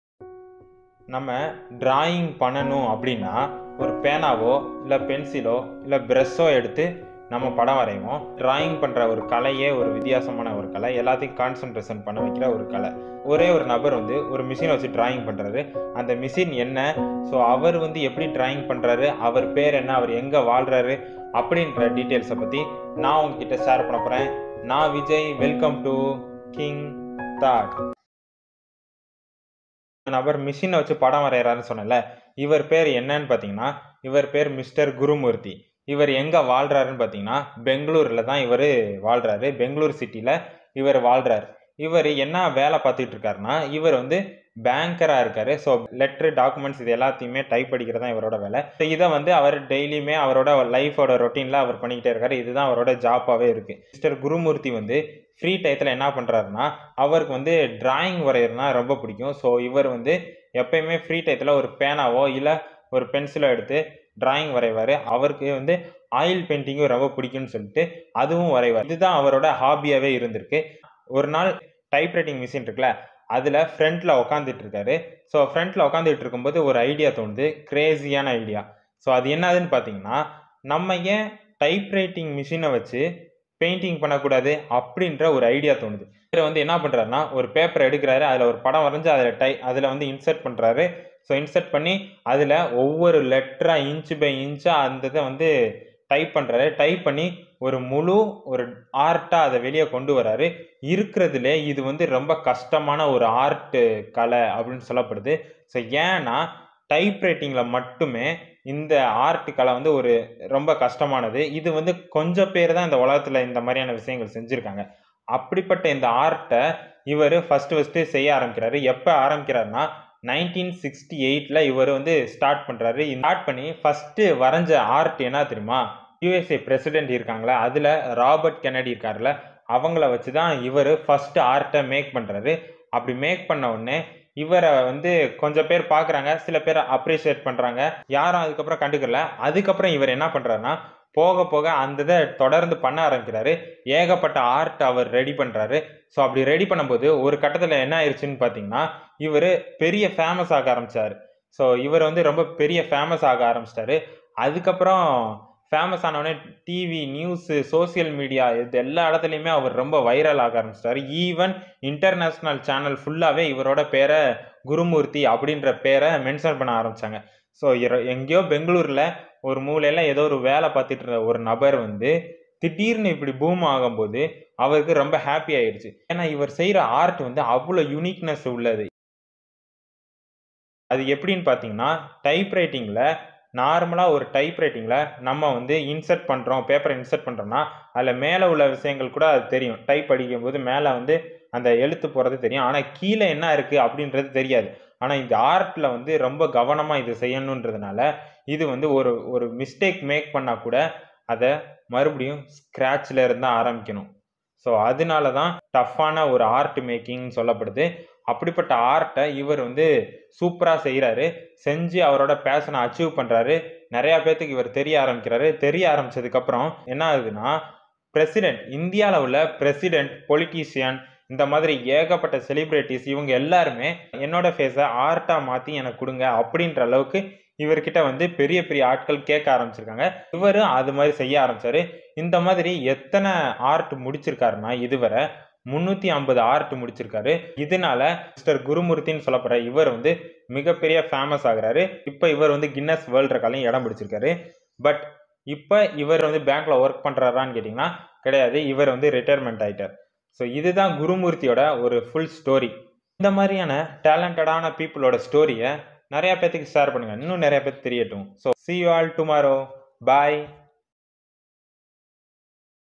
Artist of Typewriter Mr Gurumurthy History I Explain Tamil
Artist of Typewriter Mr Gurumurthy History @vjkingthought
     In this video i talk about Mr Gurumurthy Life Story .  He is the artist of typewriter . he draw for all leader face art and he living in bangalore city .  So lets me start a story .  Watch this video.